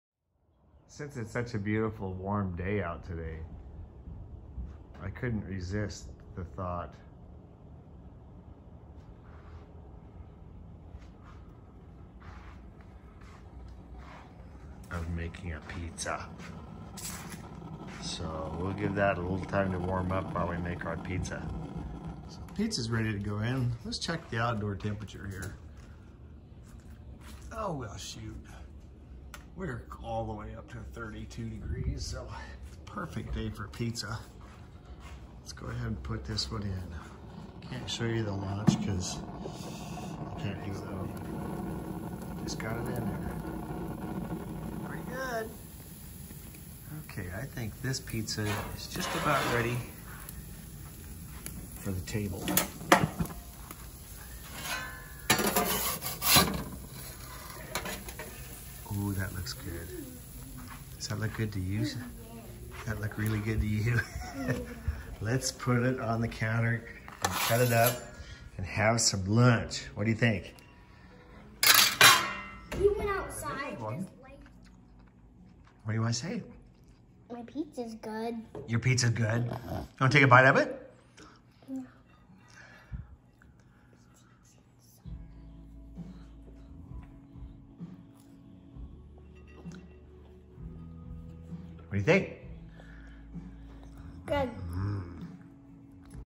Pizza on the pi at below freezing temps!
Temperature just below freezing outside. Turned the pi on (propane) and had it up to 700º in about 30 minutes. This was a particularly important cook because my 5 year old grandson was over. A very discerning pizza connoisseur that can not be fooled by ordinary store-bought pizza! It was a total success I might add.